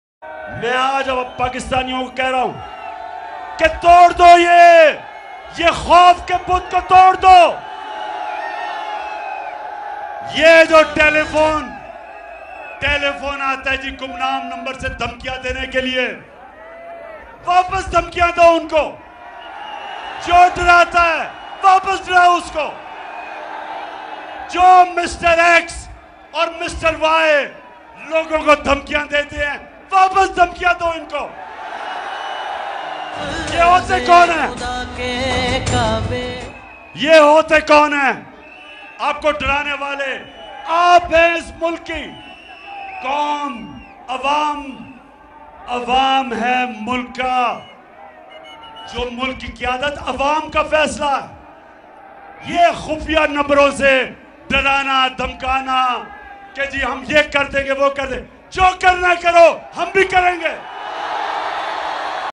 Imran Khan Speech Chakwal Jalsa | Imran Khan Most Aggressive Speech | Chakwal Jalsa Speech ImranKhan
about imran khan latest news